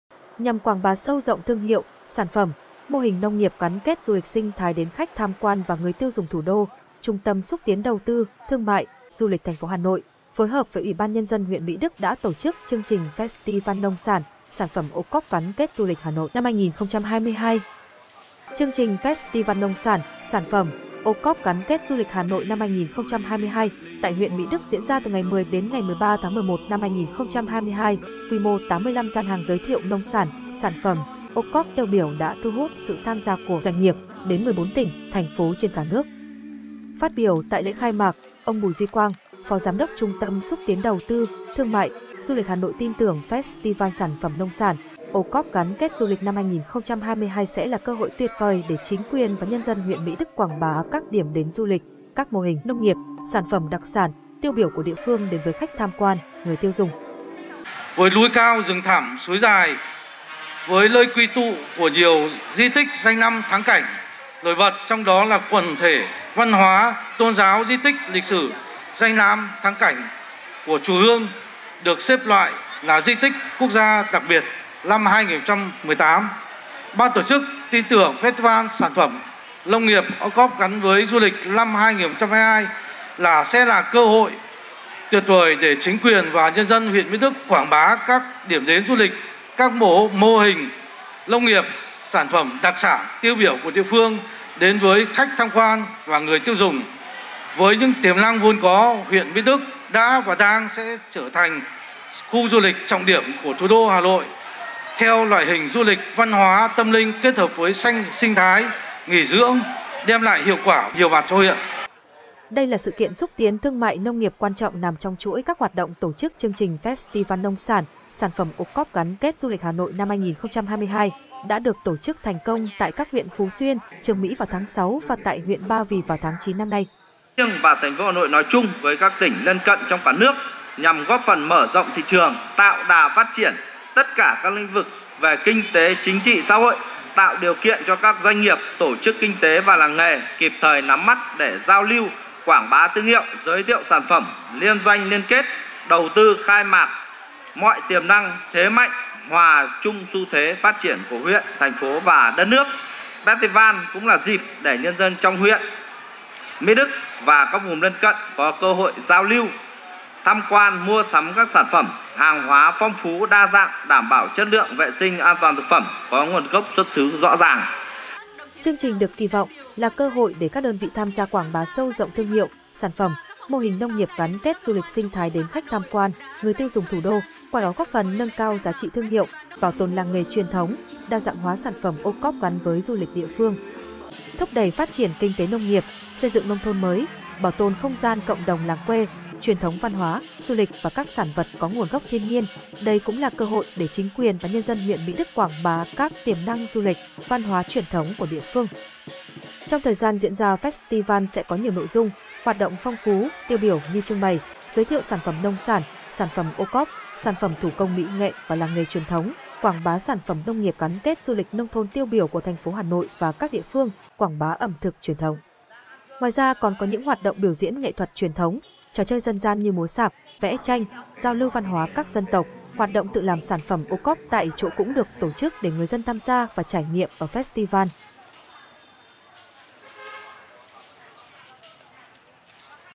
Hà Nội: "Sân chơi" giúp nông sản, sản phẩm OCOP gắn kết du lịch | Tuổi trẻ Thủ đô Online
Nhằm quảng bá sâu rộng thương hiệu, sản phẩm, mô hình nông nghiệp gắn kết du lịch sinh thái đến khách tham quan và người tiêu dùng Thủ đô, Trung tâm Xúc tiến Đầu tư, Thương mại, Du lịch thành phố Hà Nội (HPA) phối hợp với UBND huyện Mỹ Đức đã tổ chức chương trình Festival Nông sản, sản phẩm OCOP gắn kết du lịch Hà Nội năm 2022.